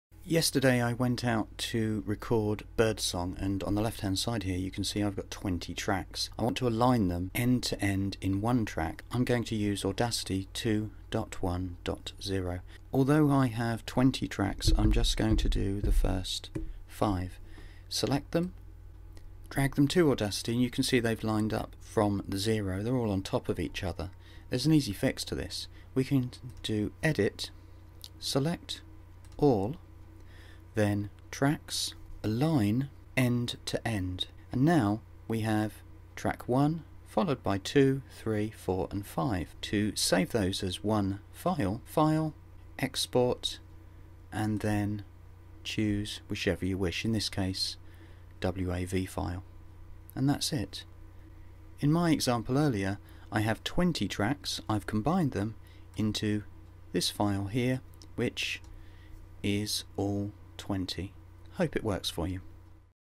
How to Align Tracks End to End in Audacity 2.1.0
I recorded 20 audio tracks of birdsong, of course I need them as one file, here's how to do it using Audacity 2.1.0 .. Audio was recorded using the Zoom H1 Handy Recorder

By combining the tracks into one file, I can edit a lot easier - for example in a video editor